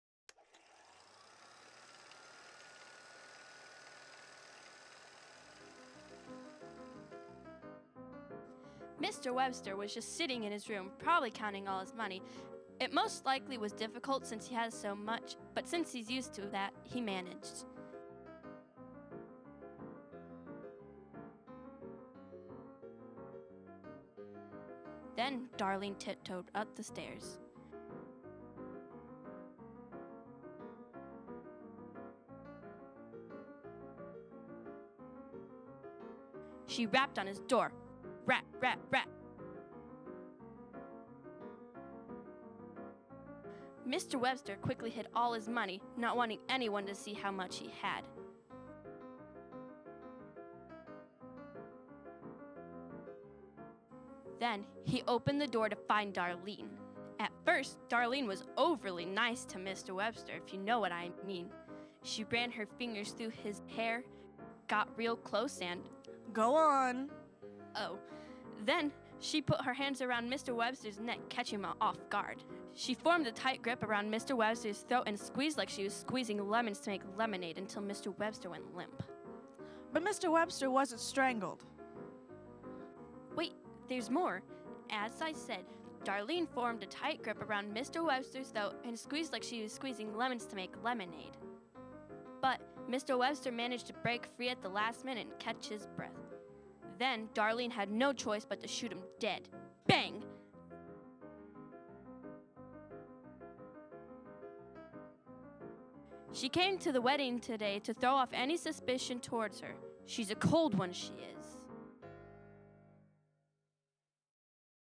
Darleen Kills Albert
Performed by the LaVille Drama Club
For the production The Groom Has Cold Feet
Filmed at Amish Acres - Fall 2017